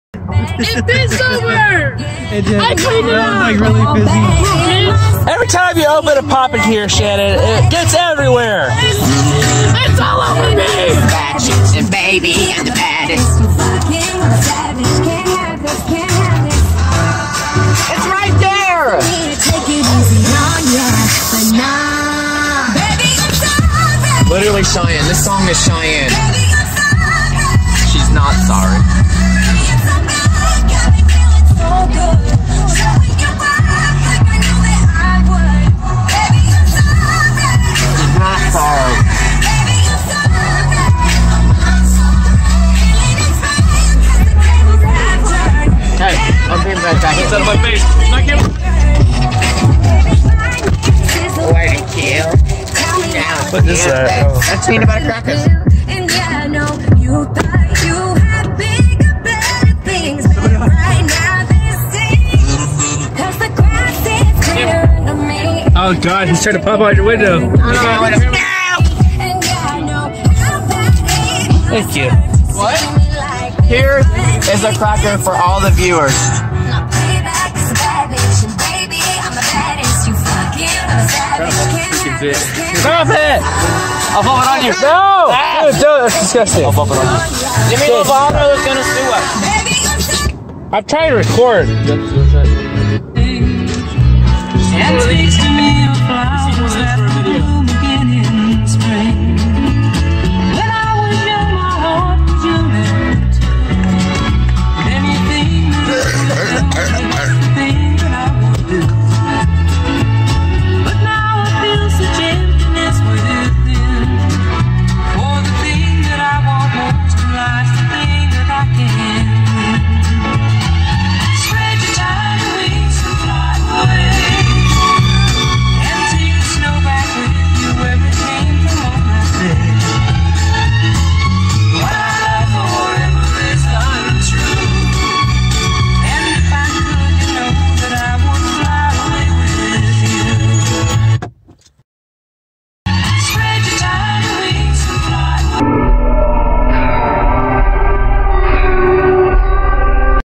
Nathan in his Car 5
The ending is the best part to this dance and music filled video of Nathan in his car.